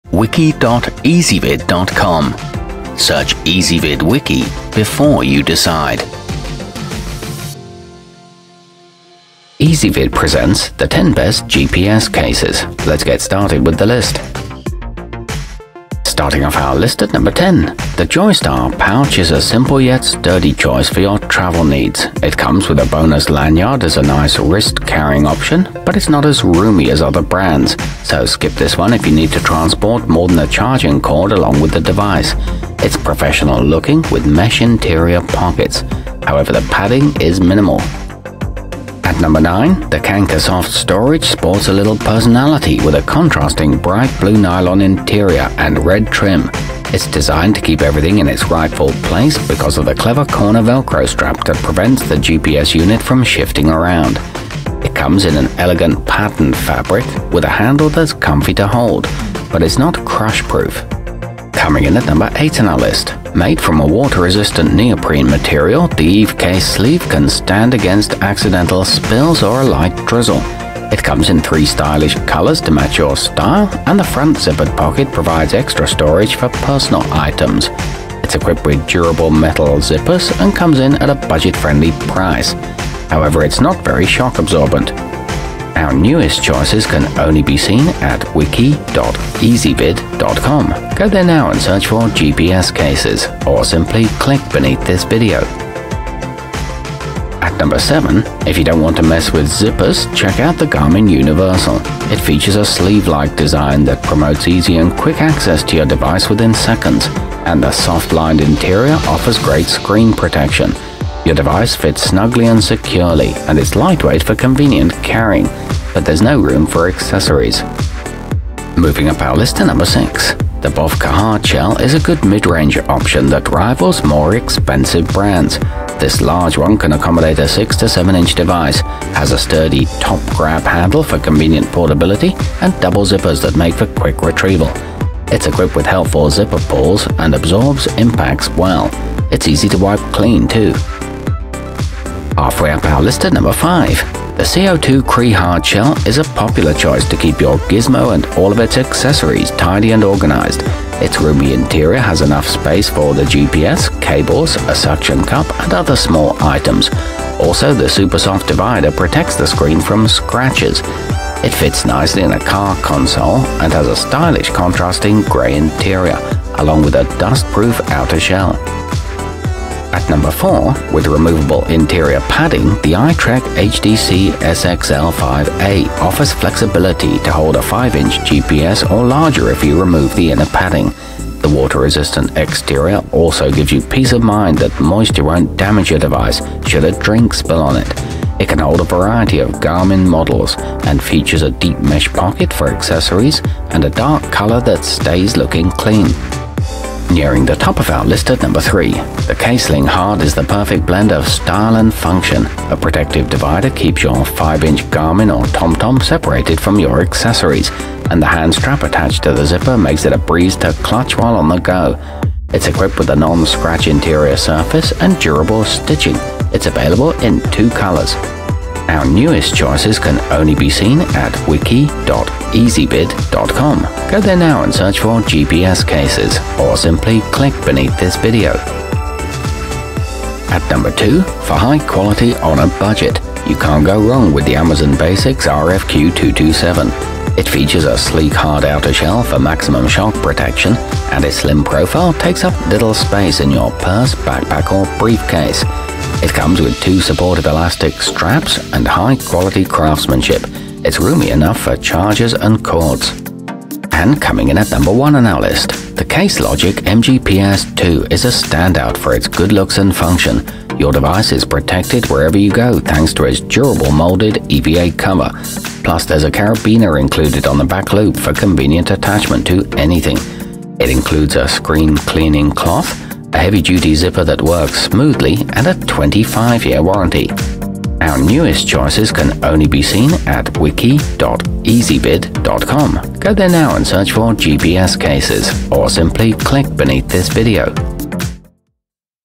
10 Best GPS Cases 2018
Our complete review, including our selection for the year's best gps case, is exclusively available on Ezvid Wiki.

Gps cases included in this wiki include the garmin universal, bovke hard shell, evecase sleeve, caseling hard, co2crea hard shell, amazonbasics rfq227, khanka soft storage, case logic mgps-2, joystar pouch, and itrek hdcsxl5a.